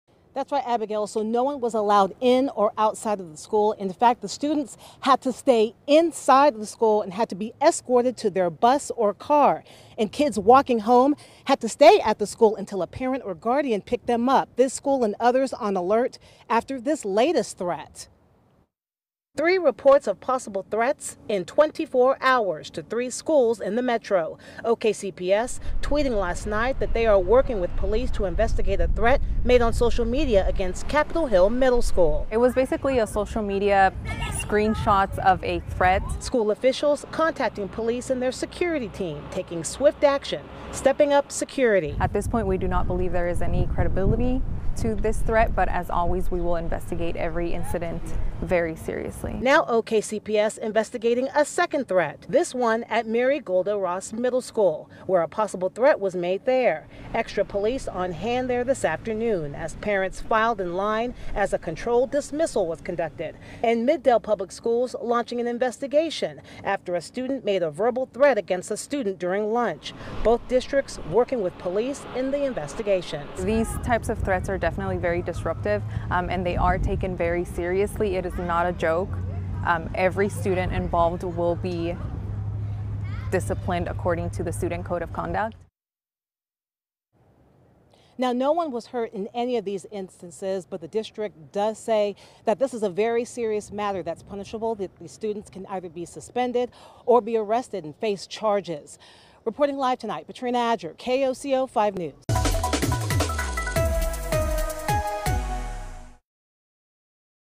Authorities investigate 3 threats made in 24 hours at metro schools
Authorities investigate three threats made in 24 hours at metro schools